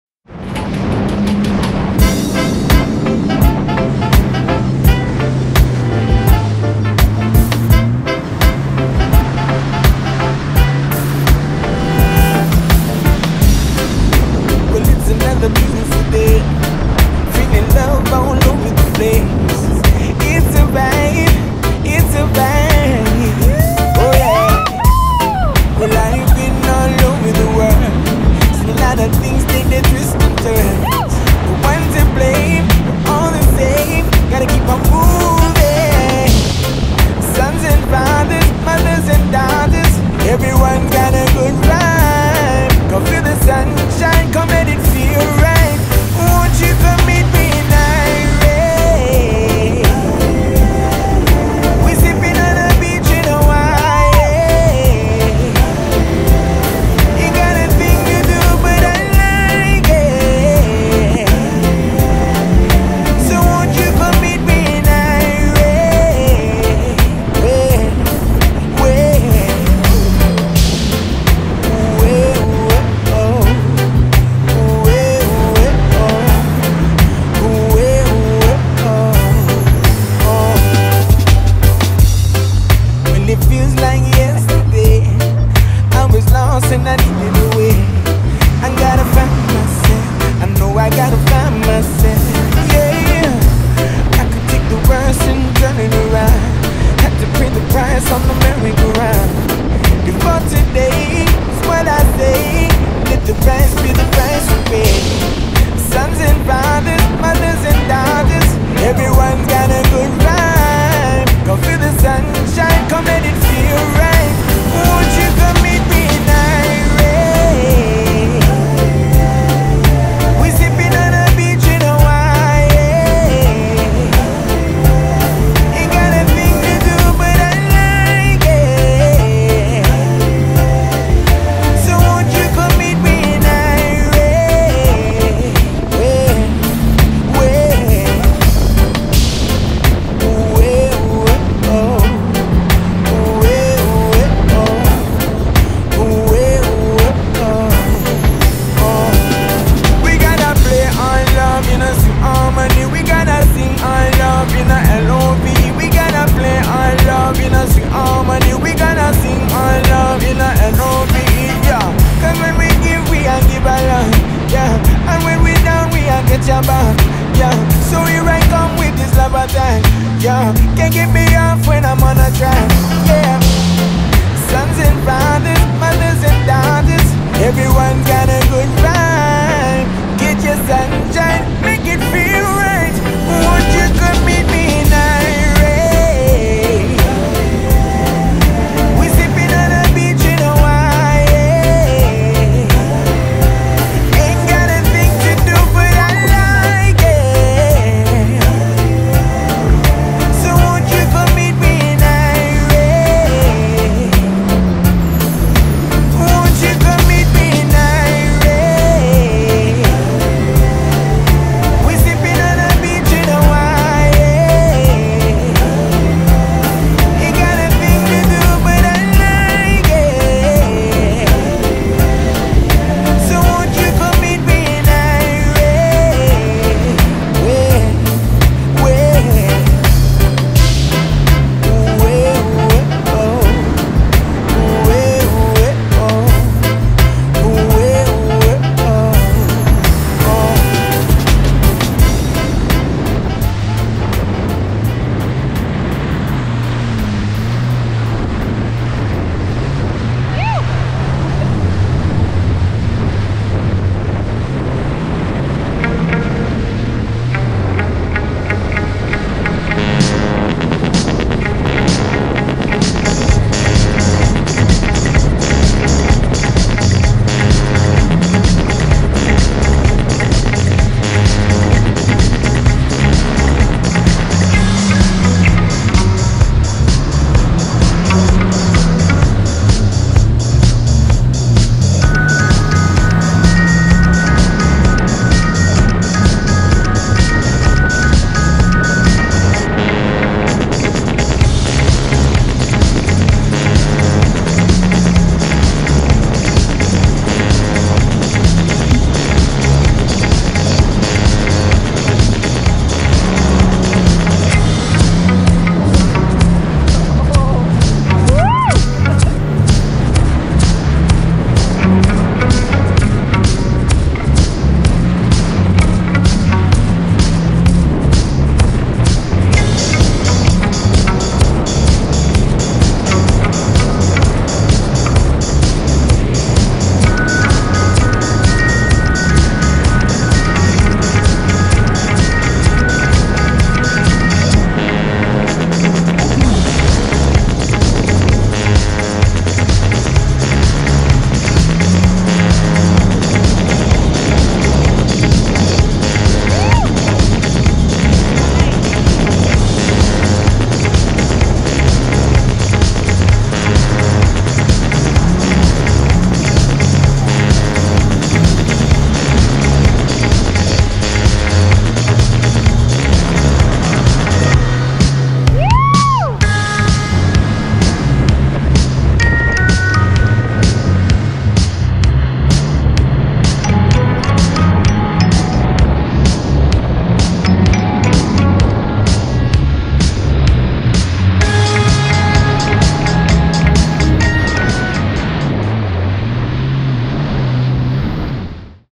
Toiling... 1st Swell of the Year, Yeeew 2022!!!
Magic Morning! Tow Foiling with Friends. Hawaii.